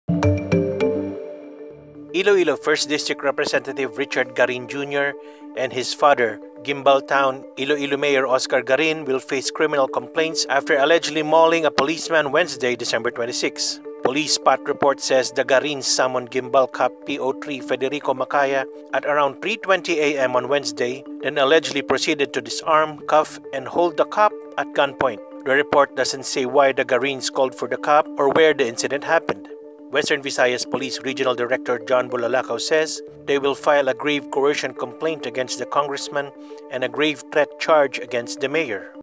PNP to sue Iloilo mayor, congressman Garin for mauling cop
More videos on Rappler Follow Rappler for the latest news in the Philippines and across the globe. Iloilo congressman Oscar 'Richard' Garin Jr and his father Guimbal town Mayor Oscar Garin also disarmed, cuffed, and held a cop at gunpoint, according to the police

Story Follow Rappler for the latest news in the Philippines and around the world.